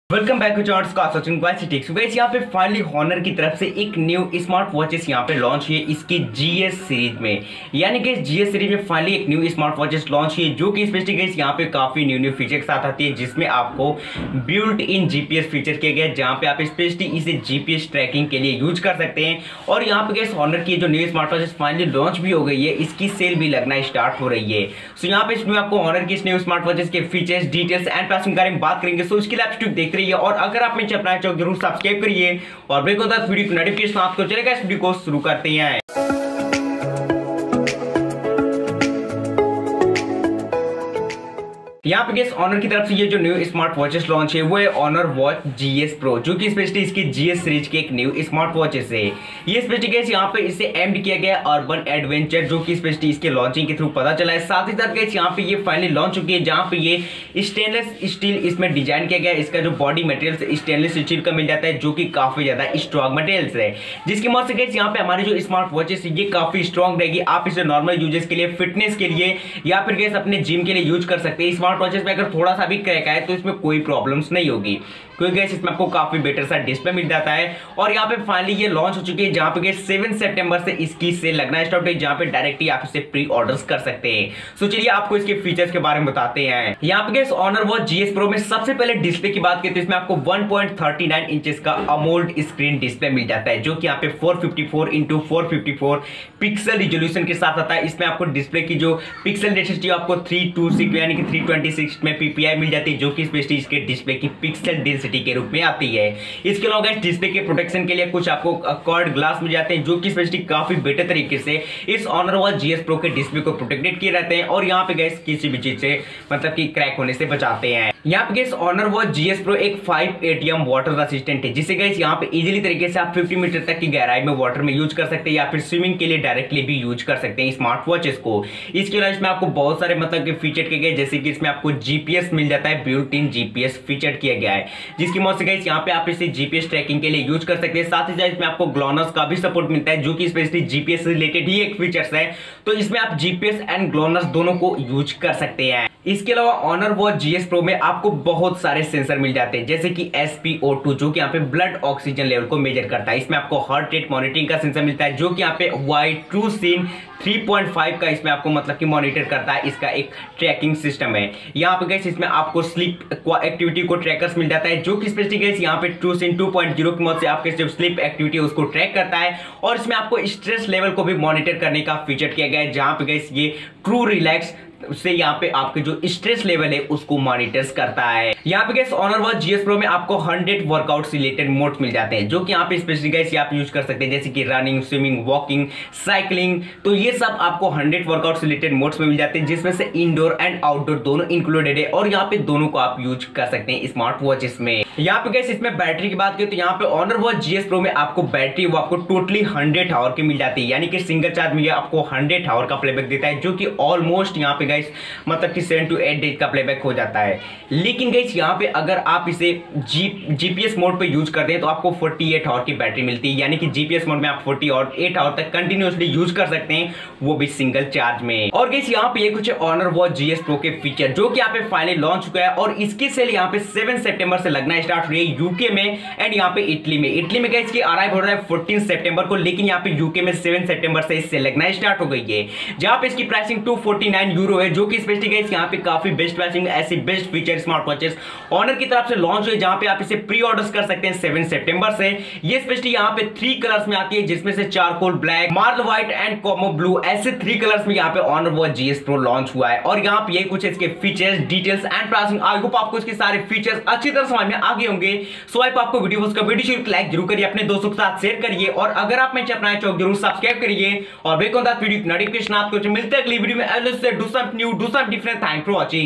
Honor Watch GS Pro | Features & Reviews | Full Details in Hindi | Built-in GPS Features & SpO2
Hai Guys Es Video Me Hm Apko Batayenge Ki Honor Ki Nee Smartwatch Launch Hua Hai Aur Ye GPS Features Ke Sath Ati Hai 🔥🔥🔥

🔭 My Tripod